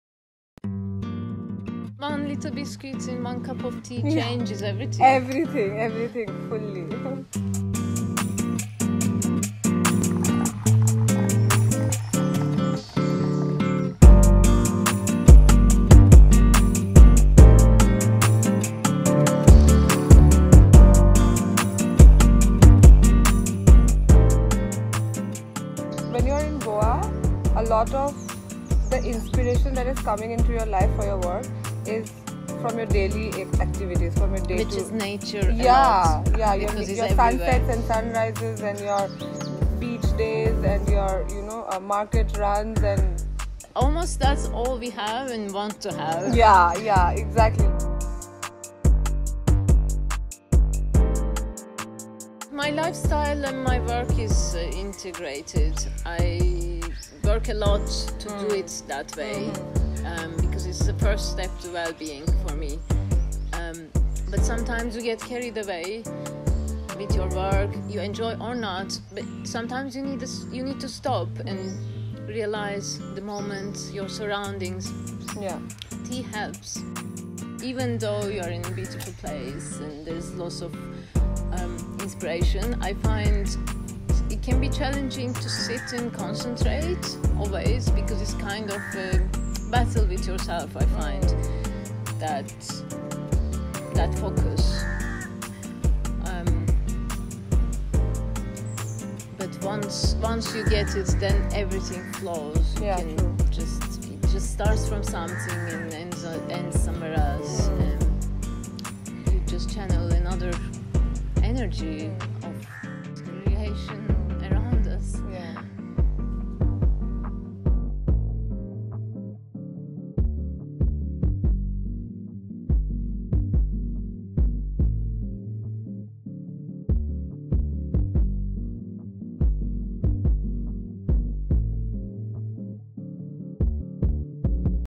The Travelling Tea Party [Episode 1]
Mixing Work and Play with Yasemin Bibin

In this episode, we have Yasemin Bibin, a Fashion Designer and Artist in conversation with Monalisa Borkakoty, a Tea Curator at Tea Amo. Over the course of a few hours and a lot of Tea, they ponder over life, art, craft, tea, and finally Yasemin Bibin photographs Monalisa in her latest collection of designer outfits.

The Travelling Tea Party is a series of short documentaries on the Tea Culture of the World. Through a dialogue over multiple cups of tea, Monalisa Borkakoty, a tea curator at Tea Amo, speaks to people from around the world about their lives, their craft, and the importance of a good cup of Tea.